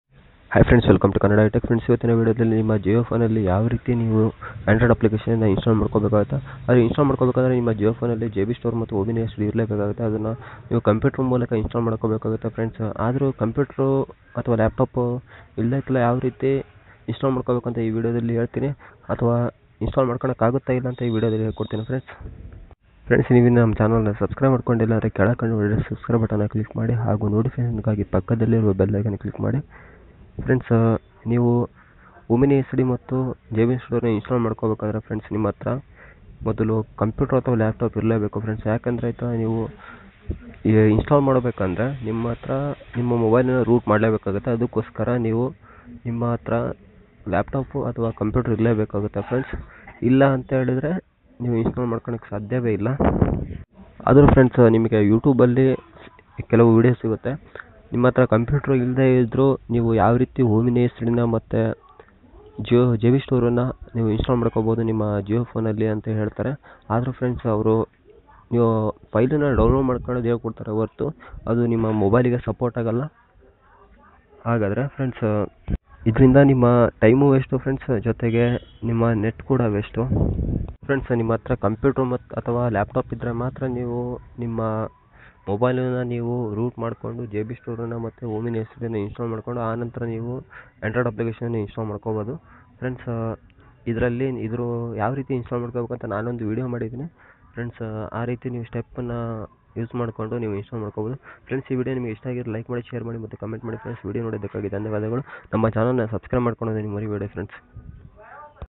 Download omnisd for jio phone in kannada | download android app for jio phone in kannada
Flipkart My Geares

Smart watch Hi ನಮಸ್ಕಾರ
                      wellcome to kannada i tech

  in this video i tell about omnisd app | in  jio phone with out root we cannot instal any app